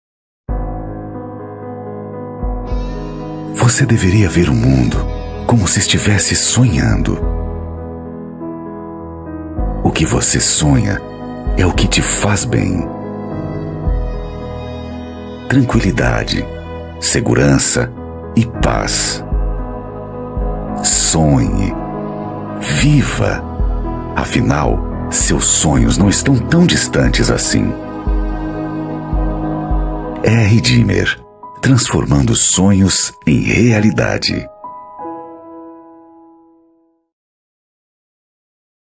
R Dimer Transformando Sonhos em Realidade
A experiência e um olhar apaixonado por detalhes remontam a história da R. Dimer Empreendimentos. A R. Dimer Empreendimentos acredita que é o somatório de profissionais qualificados, a constante busca por tecnologia e processos que dão forma aos seus projetos. Elegendo Três Cachoeiras e Torres, como palco para suas obras, a R. Dimer eleva o conceito de viver com sofisticação e investir com qualidade, afinal, estar cercado de belezas naturais por todos os lados é mais que um convite à inspiração: é um privilégio.